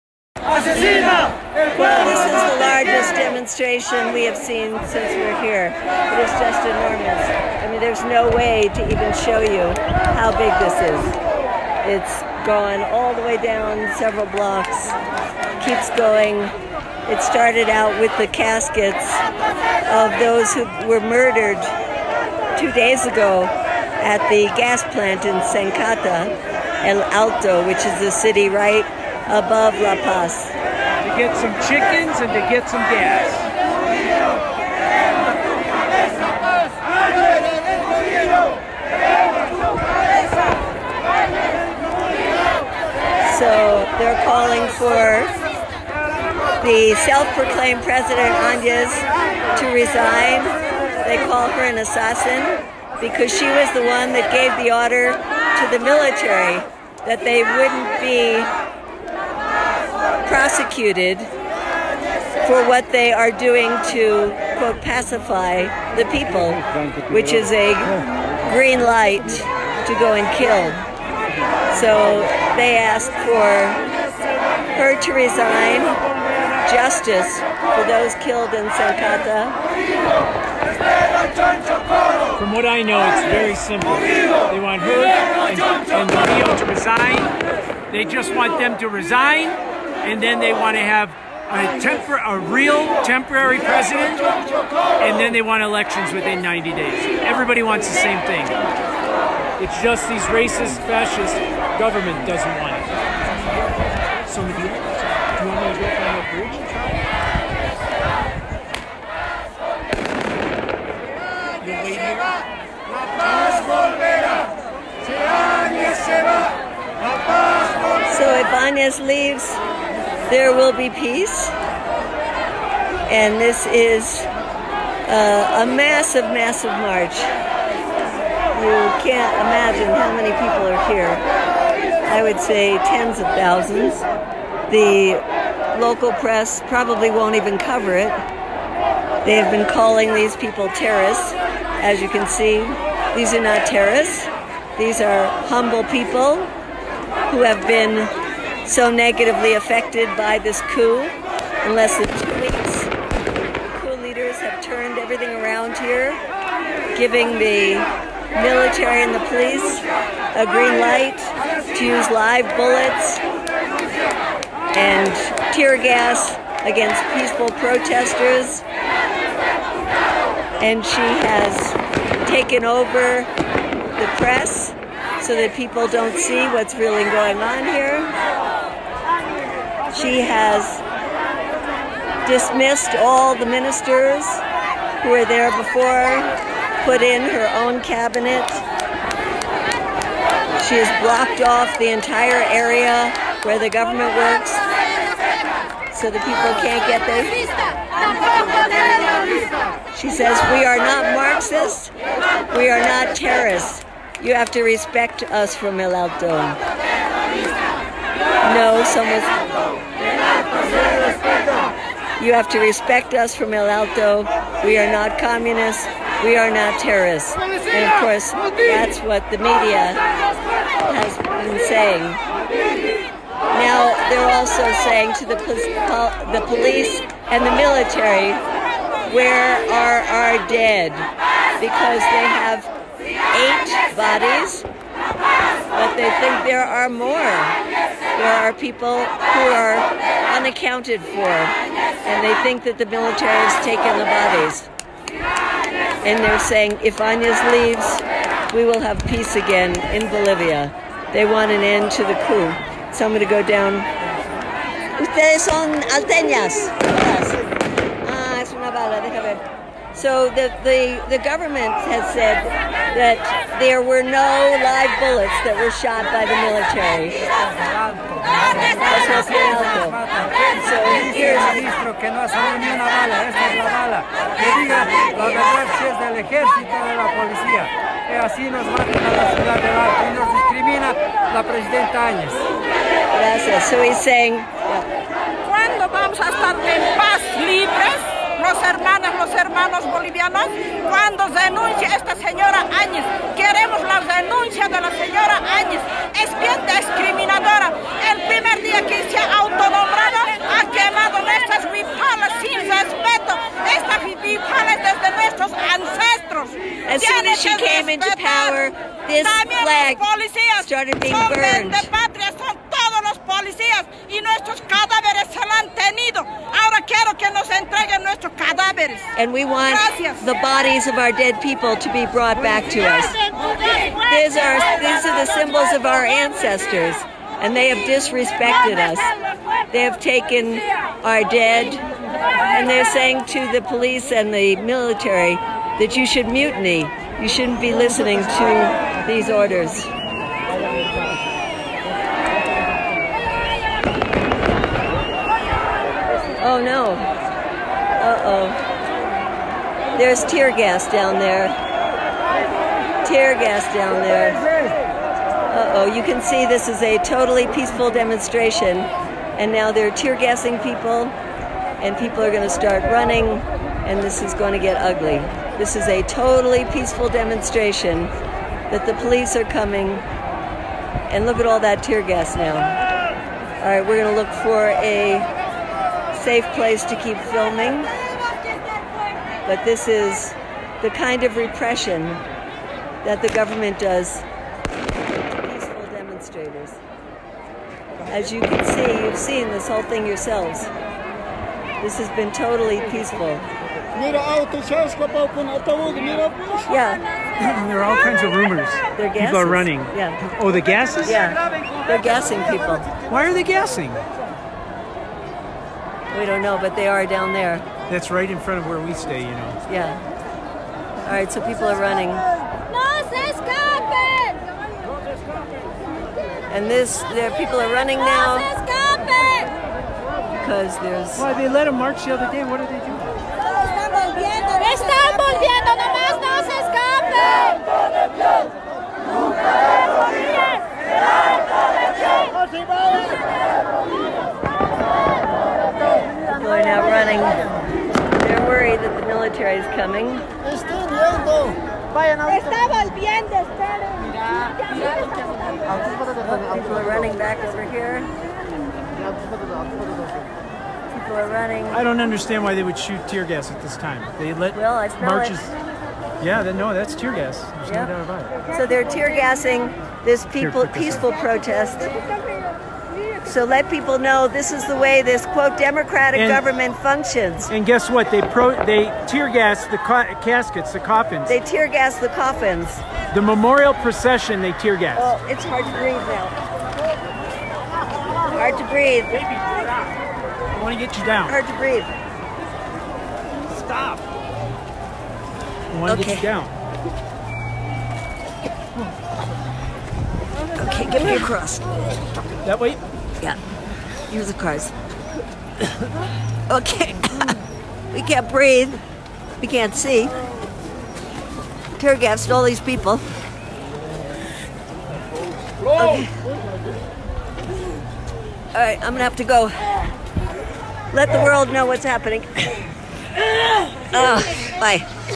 Bolivia: Coup-backed forces gas peaceful protesters
Medea Benjamin captures the moment when coup-backed military and police forces tear gas peaceful protesters as they march in honor of those murdered at Senkata. Protesters did absolutely nothing to warrant the use of tear gas. This is repression.